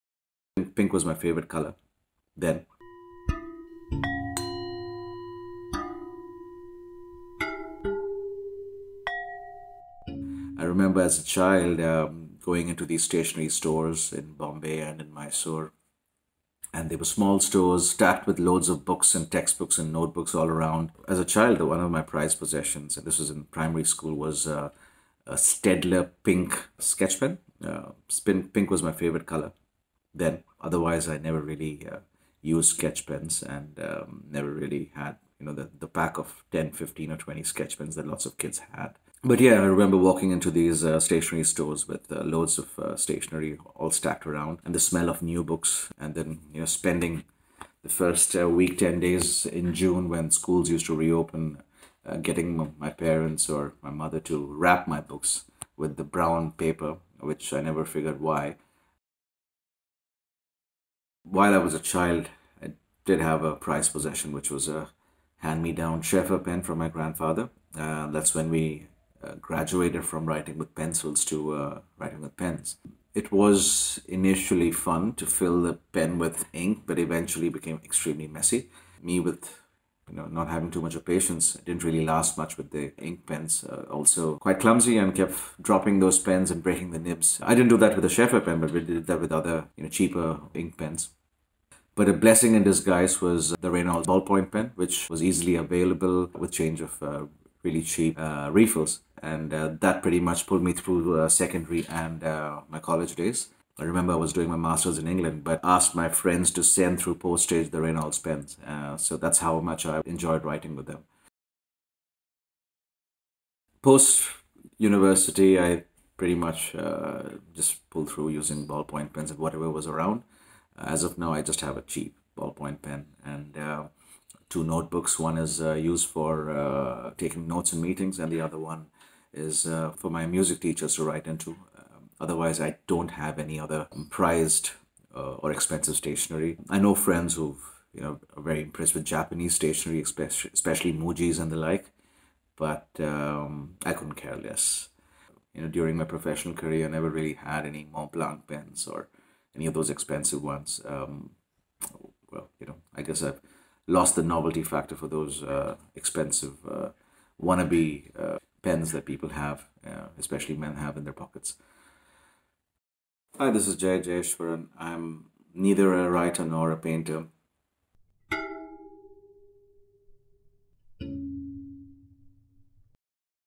Expensive Fountain Pens VS Cheap Ballpoint Pens - Stationery Trails Shorts with Jay Jayeshwaran
There are all kinds of stationery lovers out there, ranging from the stationery addicts to the minimalists. Jay Jayeshwaran is a finance professional based in Adelaide, Australia. He spoke to Inky Memo about why he has never felt compelled to buy branded luxury fountain pens like his peers and relies on the trusty Reynolds ballpoint pen. We detect a minor RANT in the video! Watch it and enjoy.

1.  Sheaffer Intensity Carbon Fiber Fountain Pen
2. Staedtler Double-Ended Fiber-Tip Pens
3. Set of 5 Pens Reynolds 045 Fine Ball Point Pen
4. Montblanc Meisterstuck Classique Black Rollerball Pen
5. MUJI Smooth Gel Ink Ballpoint Pen

Stationery Trails Shorts is an an exchange of stationery experiences and memories. Think of it as friends discussing their favourite stationery stories over a cup of tea, muffins and wide smiles!

We are Inky Memo

Timestamps:
00:08 Tell us about your stationery store memories as a child.
01:00 What's a specific piece of stationery that's important to you?
01:52 Confessions of a stationery minimalist.